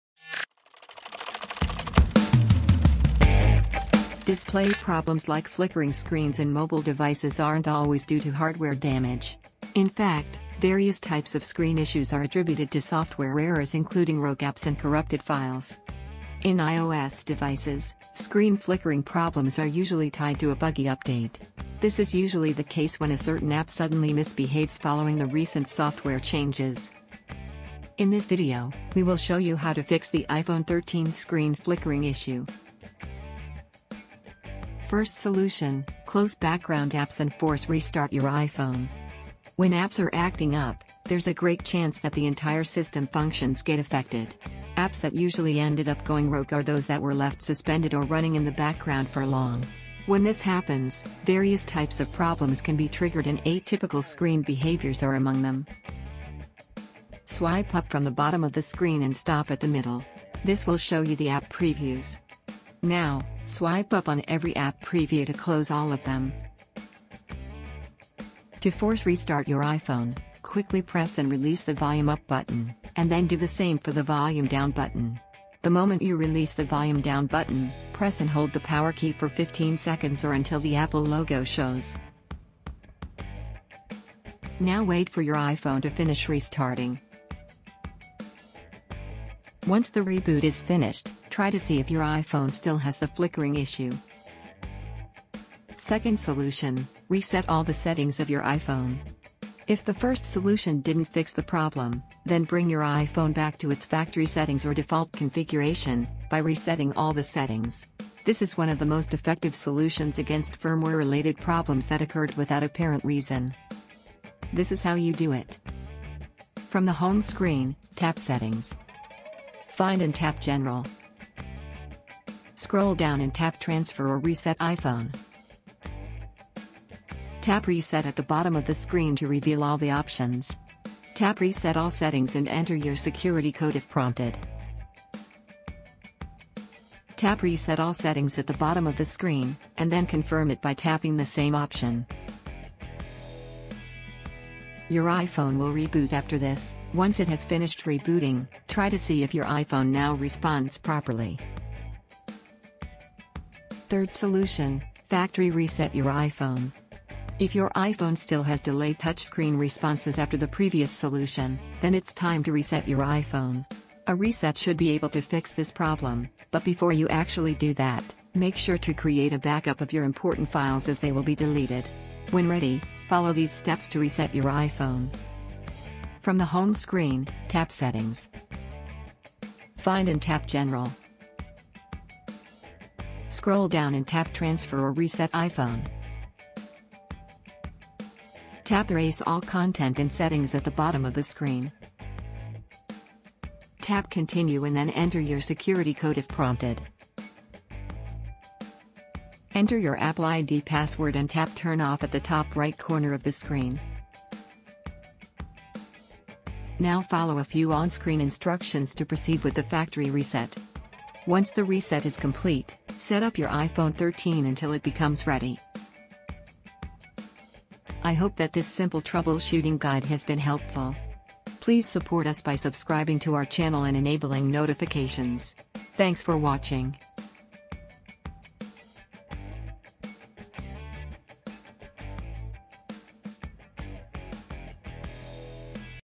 How To Fix Apple iPhone 13 Screen Flickering Issue (iOS 15)
Display problems like flickering screens in mobile devices aren’t always due to hardware damage. In fact, various types of screen issues are attributed to software errors including rogue apps and corrupted files.
In iOS devices, screen flickering problems are usually tied to a buggy update. This is usually the case when a certain app suddenly misbehaves following the recent software changes.

In this video, we will show you how to fix the iPhone 13 screen flickering issue.
0:00 Introduction
0:36 First solution: Close background apps and force restart your iPhone
1:37 Second solution: Reset all the settings of your iPhone
2:36 Third solution: Factory reset your iPhone